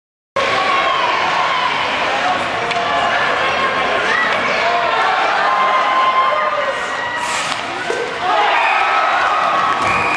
Lance Lucas scores goal 3 vs Southgate
Lance Lucas scores short handed to put South up 3 to 0 vs Southgate